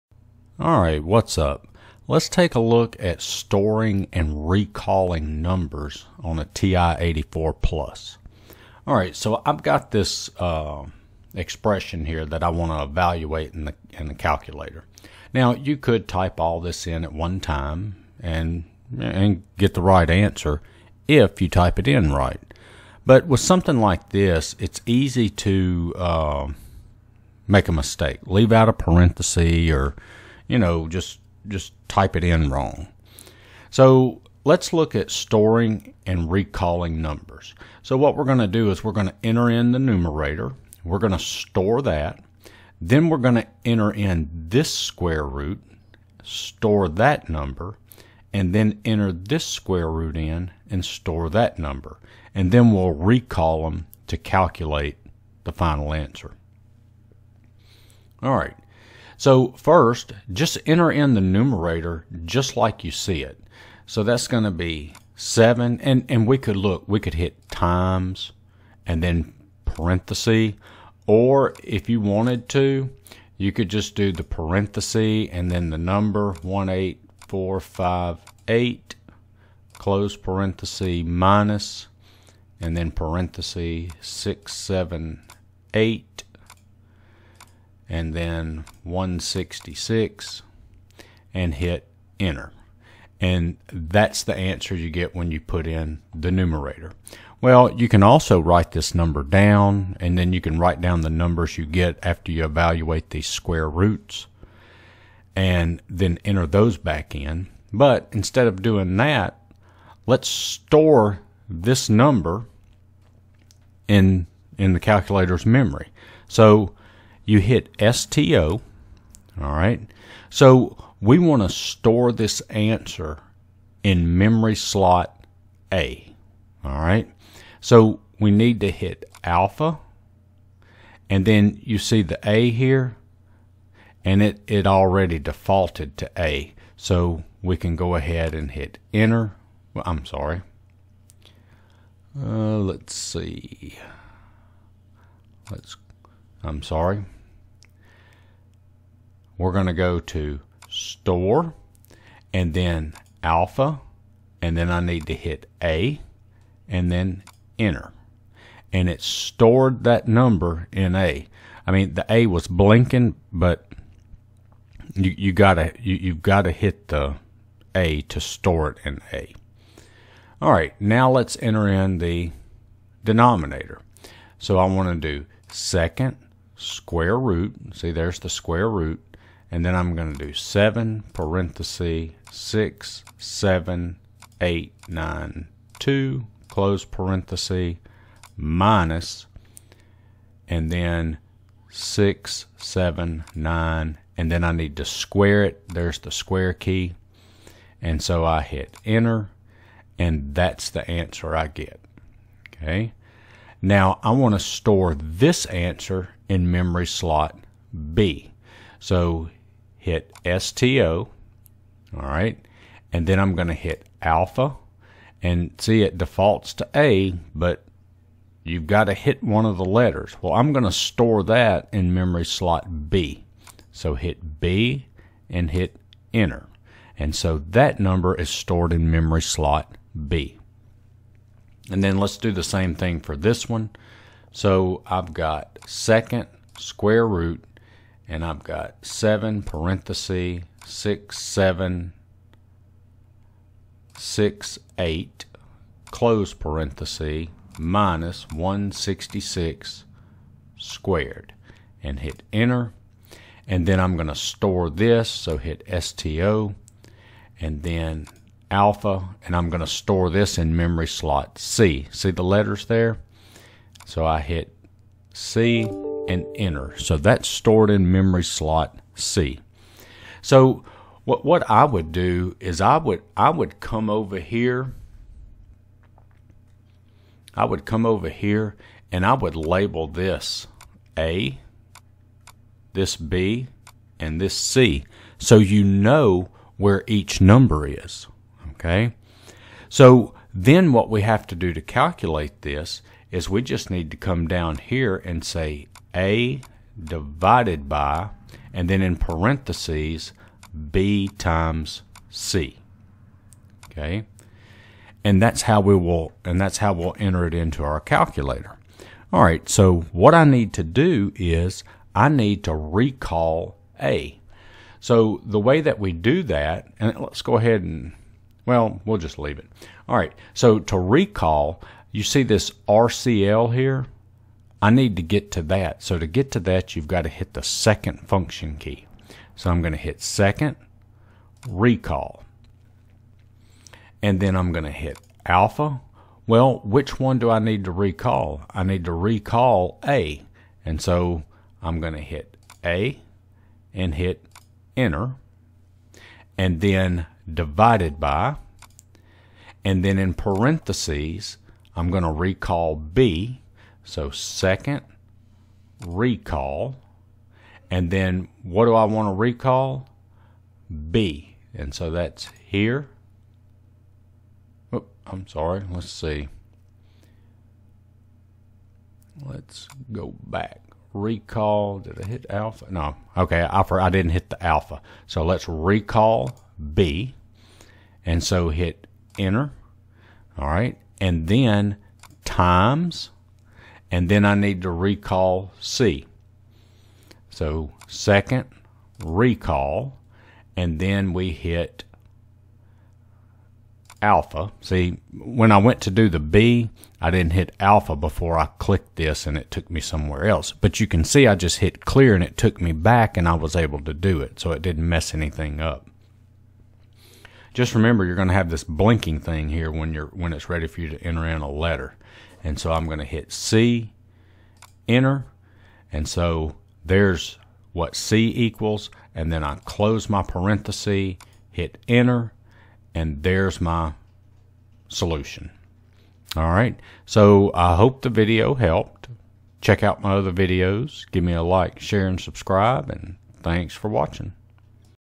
TI-84 Plus How To Store And Recall Answers
This video shows how to store and recall answers on the TI-84 Plus